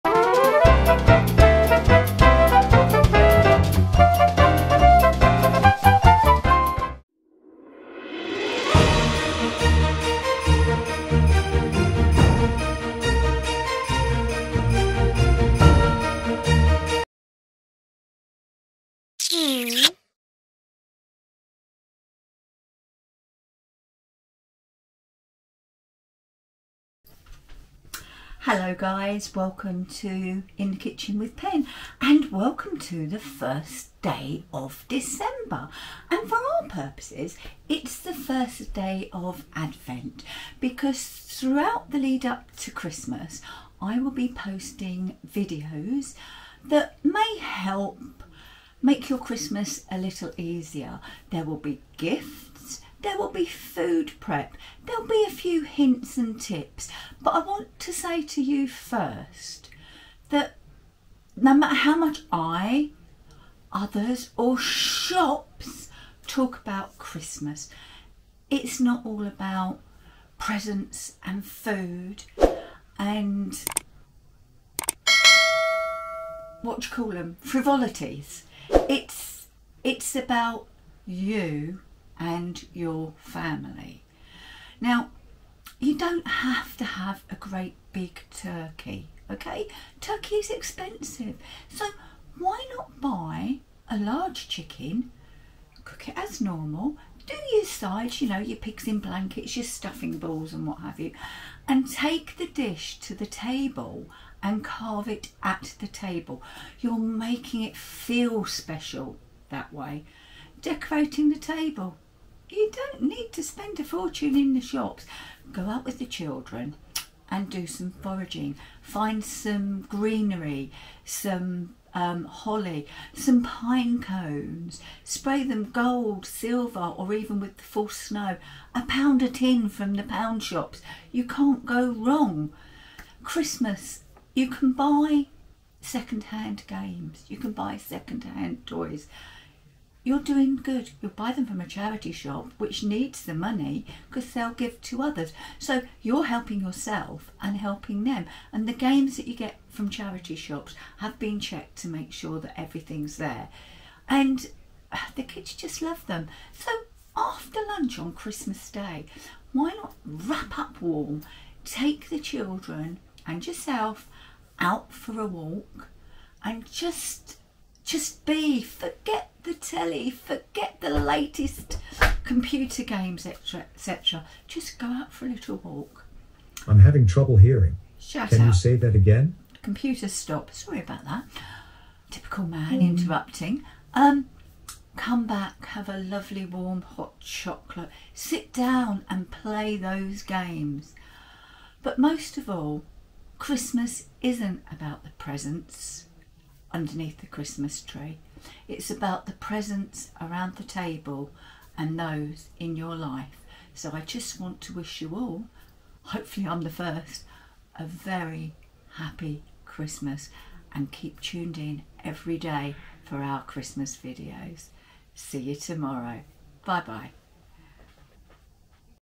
The first day of our Christmas advent . 24 days of recipes hints and tips
Welcome to in the Kitchen with Penn (& Steve) at Christmas.

We'll be sharing make ahead recipes for your Christmas Dinner, cakes and bakes .
Lots can  be made with the children

 So sit back and enjoy Stepping into Christmas with us